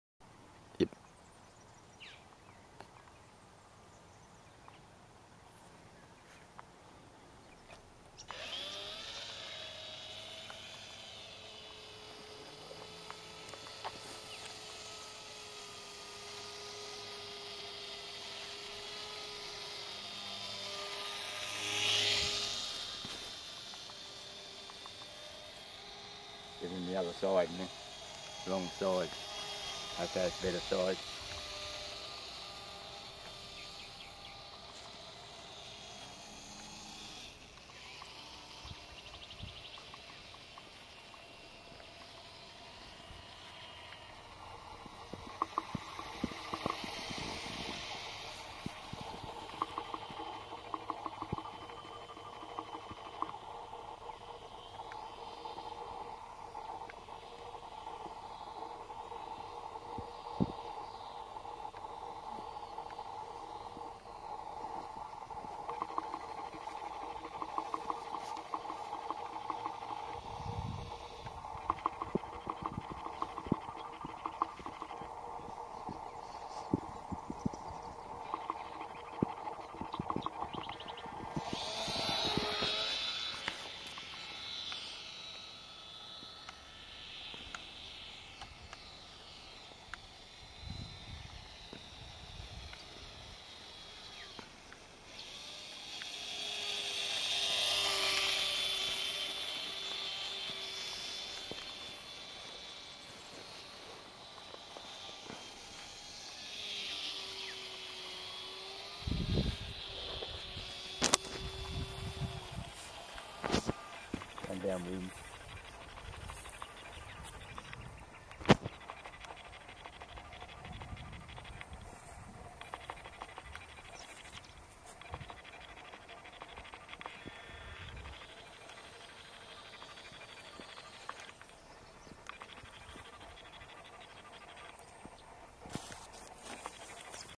RC SLOW FLYER (Made From the Popular Cessna 182)
SLOW FLYER MADE FROM THE CESSNA 182.
It's the cheap Cessna 3 ch RC plane (GT 4130). Cost about 70 Australian dollars. A popular plane and great value for money. I bought 3 of them, kept one original and changed the other 2.